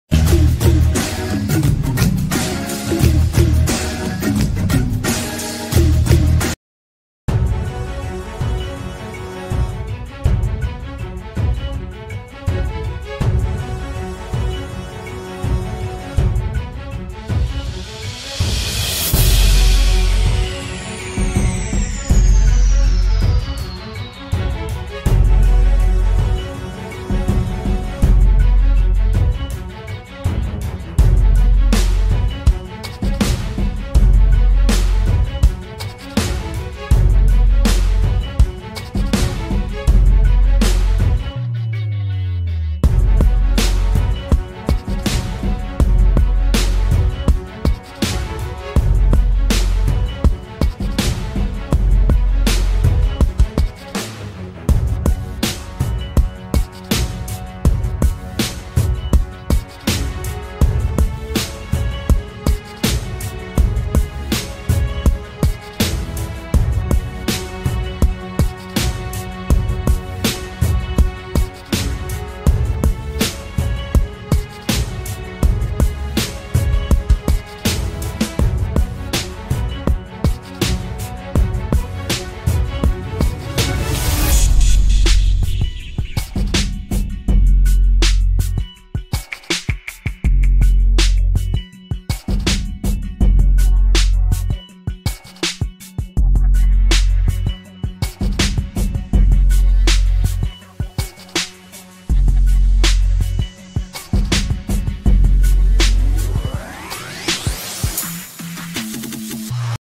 Kebakaran Terjadi Di BEC Bandung
Bandung Electronic Center (BEC) pada malam ini Kamis 18 Februari mengalami musibah kebakaran, belum diketahui penyebab kebakaran tersebut.